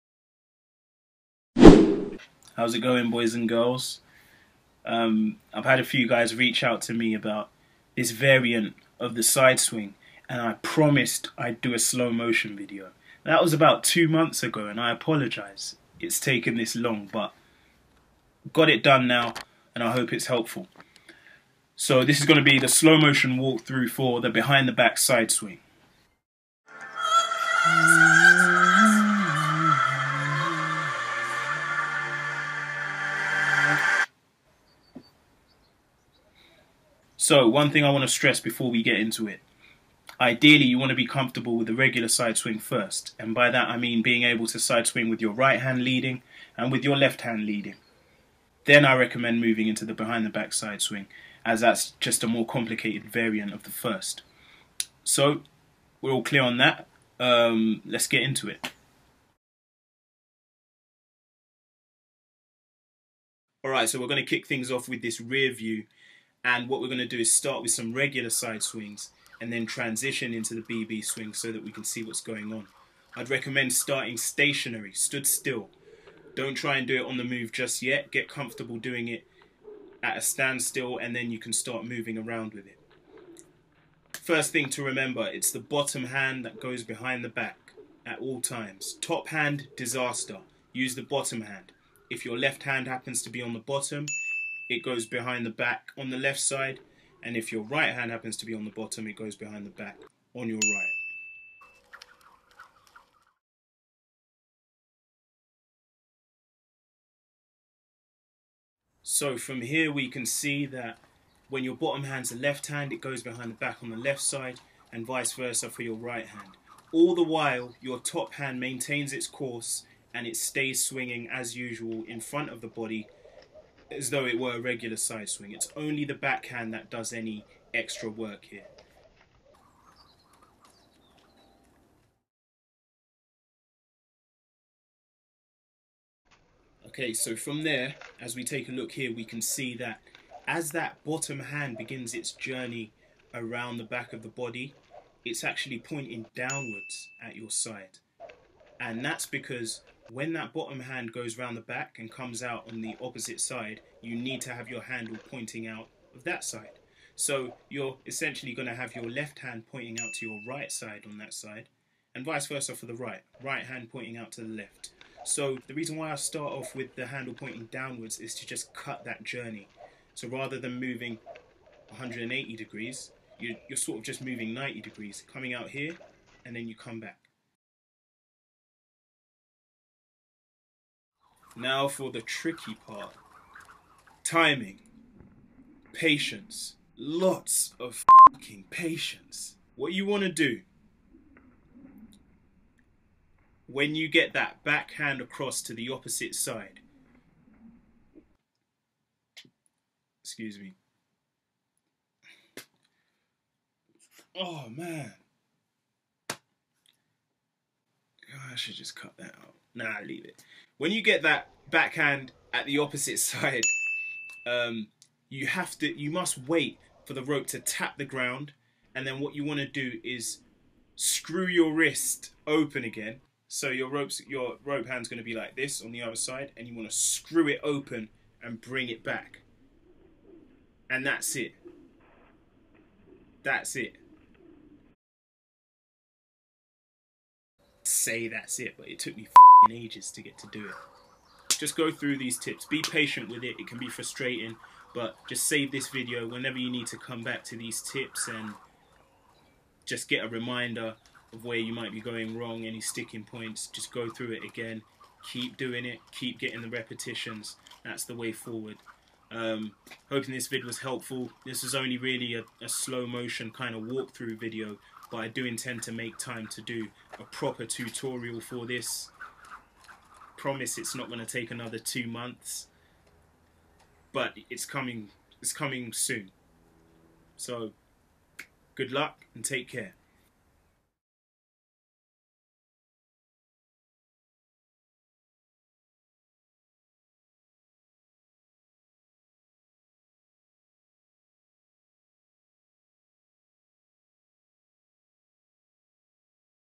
Breaking Down The "BB Sideswing" Technique | JUMPING HACKS (Jumprope Tutorial)
SUMMARY
1. Bottom hand goes behind the back (left hand via your left side; right hand via your right)
2. Top hand maintains its normal course
3. Point handle downwards at your side
4. Bring hand behind back and point handle outwards at opposite side
5. Wait for the rope to tap the ground
6. Then screw the wrist...
7. ...as you bring your hand back to its regular side.